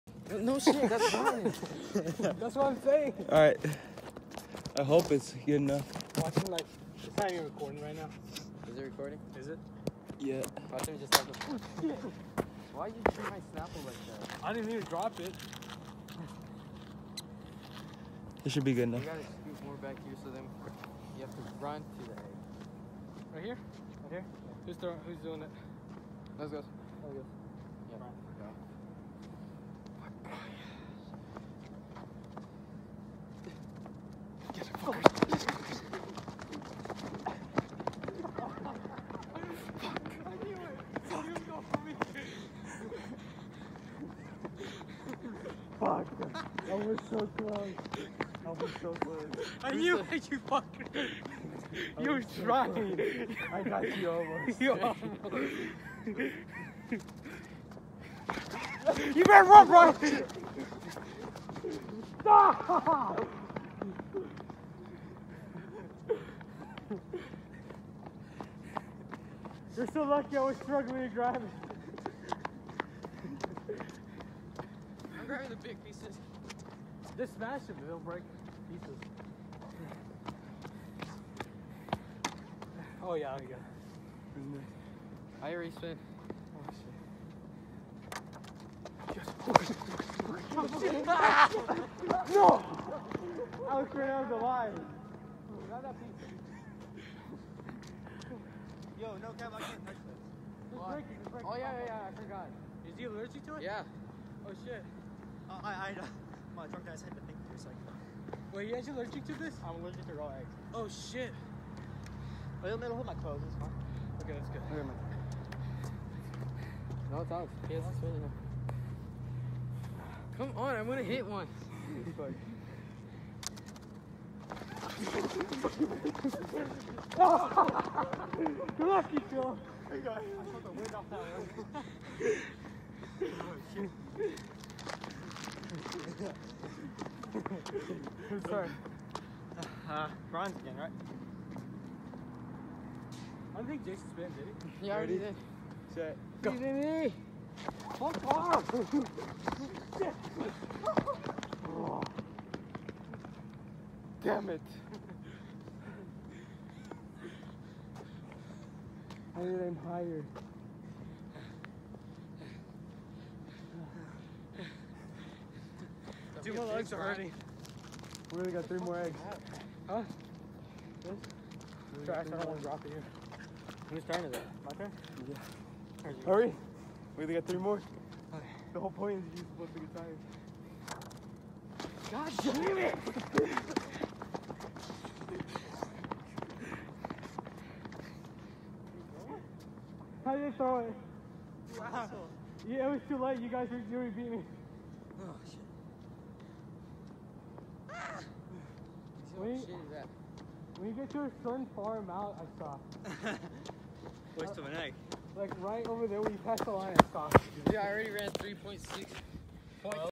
Egg Roulette with the boys
The boys and I played Egg Roulette! More to come!